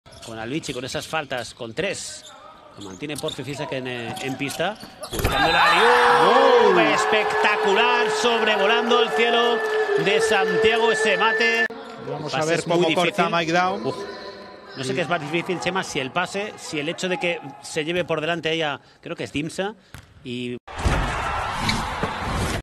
Vuelo sin motor de MIKE DAUM | Liga Endesa 2020-21
Mike Daum no podía encontrar mejor cómplice para machacar que Albert Oliver. El base lanza el alley oop, seguro de que su compañero la recogerá en el aire para machacar, a pesar de la oposición de Costello. ¡Espectáculo en el Fontes do Sar!